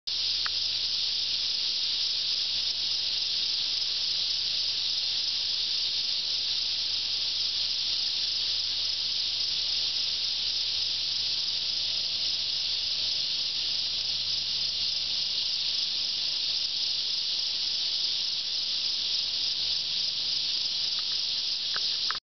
Deer on the Bike Trail
Two deer seen along the Great Miami River bike trail near Dayton, Ohio.